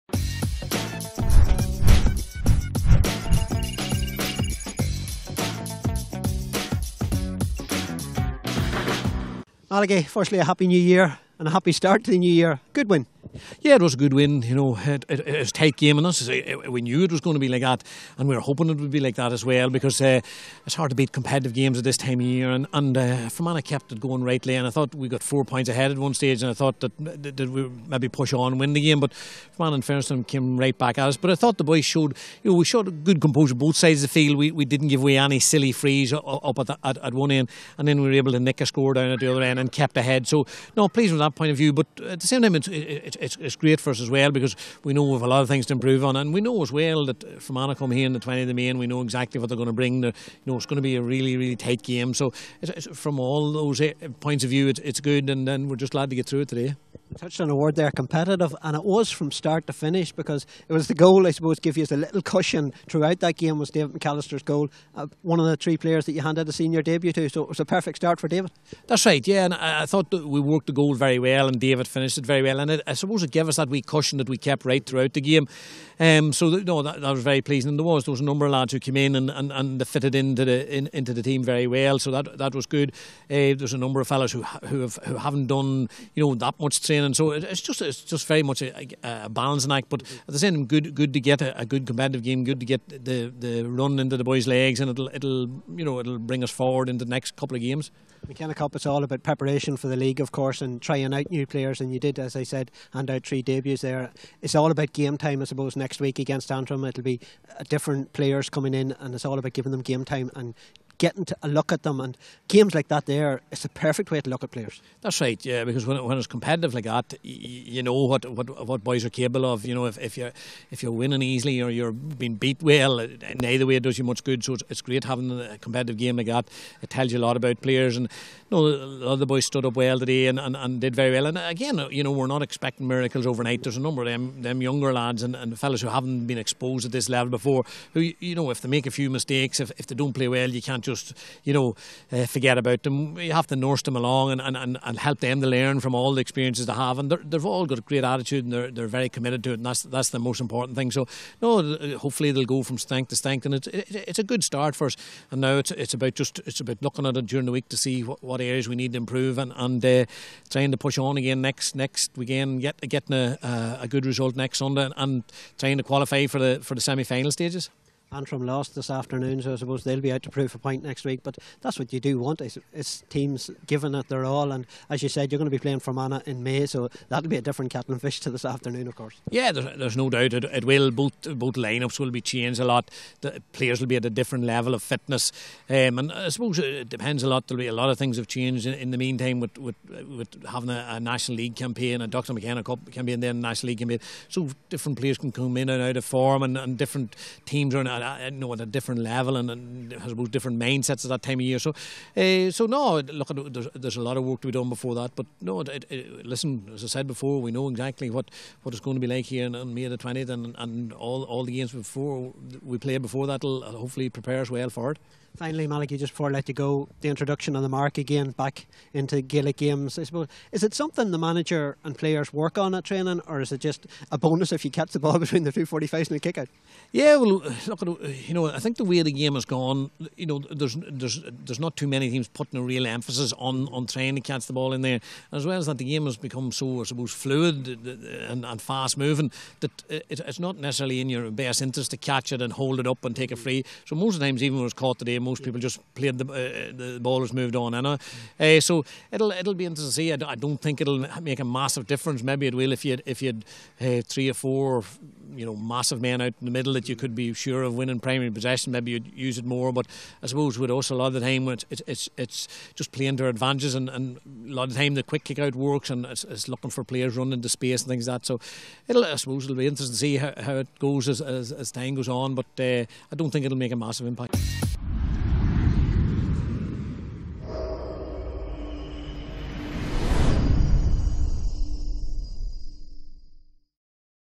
BOI Dr McKenna Cup Monaghan v Fermanagh Post Match Interview
Alan Gunn talks to manager Malachy O Rourke  in the aftermath of the Monaghan's 1-13 to 0-15 victory over Fermanagh  in the BOI Dr Mc Kenna Cup Rd 1 Game held in Clones on 8/1/16